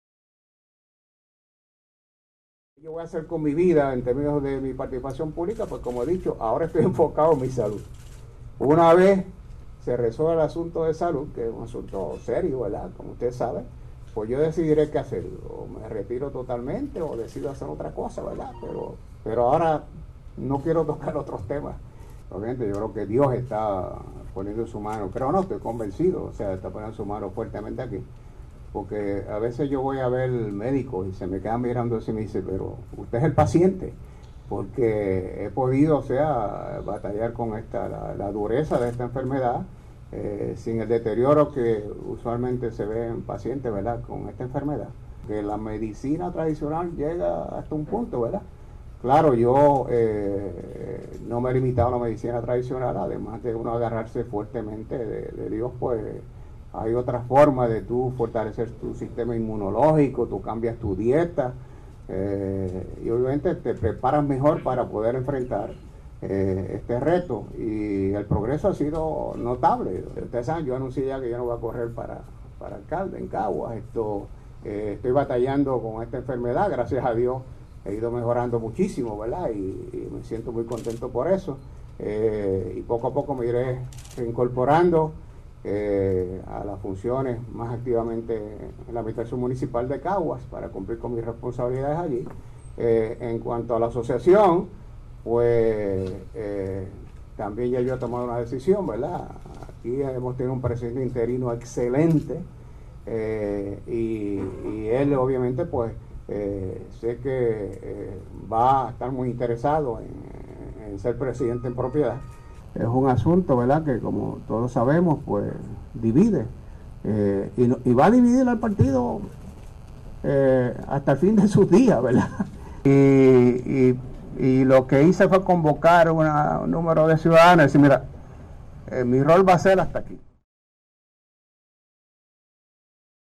William Miranda Marín en reunión de alcaldes
El alcalde de Caguas, William Miranda Marín, habla de su batalla contra el cáncer y de su futuro político. El alcalde Willie Miranda Marín aún no ha determinado cuál será su futuro político una vez culmine su mandato en el municipio de Caguas. Por ahora está enfocado en vencer su condición de cáncer de páncreas.